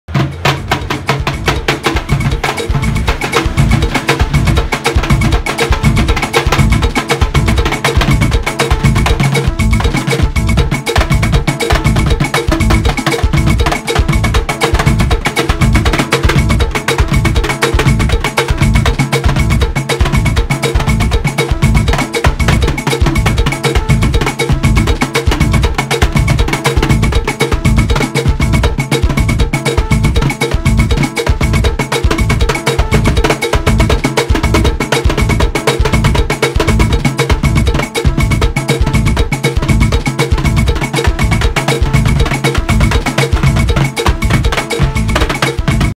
McKinsom Percusión #1 2020
VideoProd:         Photo. G.E
Percusión:          McKinsom
Lugar:  Puerto Barrios, Izabal, Guatemala 🇬🇹